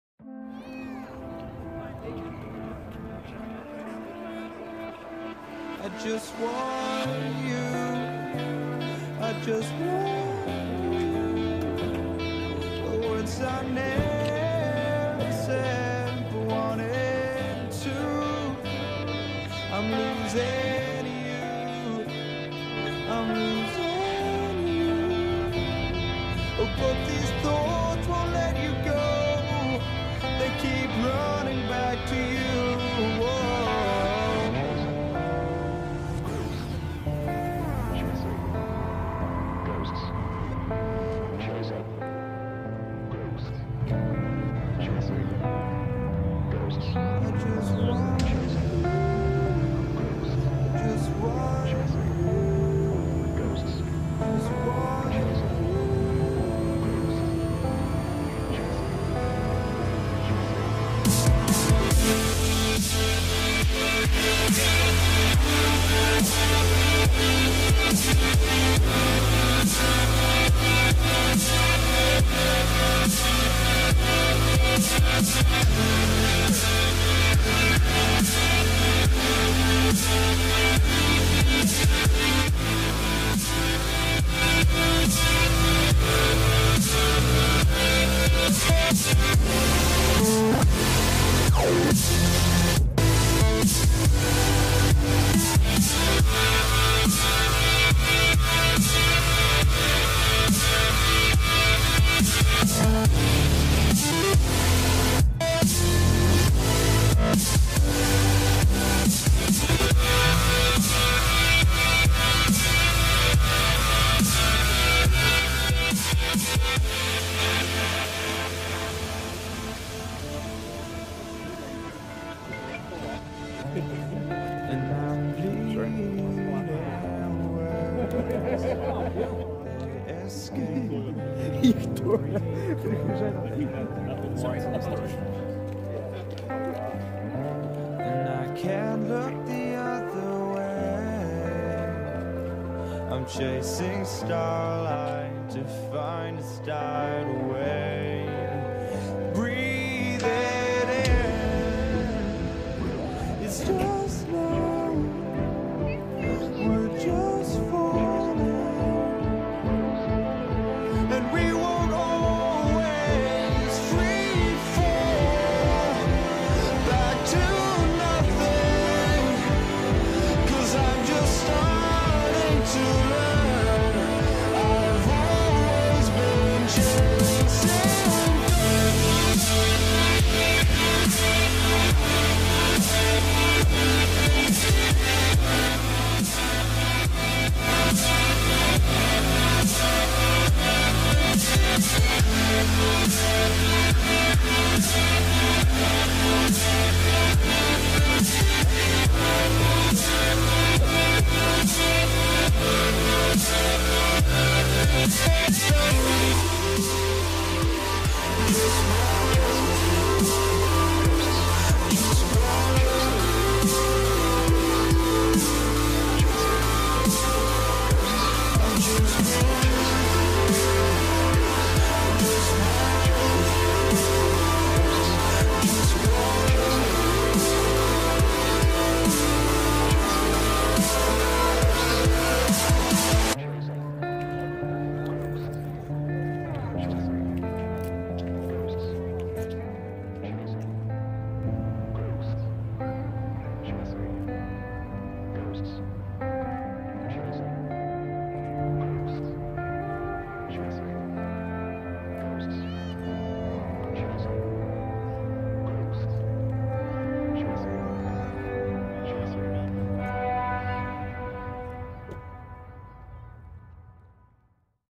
Cars & Coffee, Langley B.C.
Cold and wet winter morning. Cars and Coffee meet up.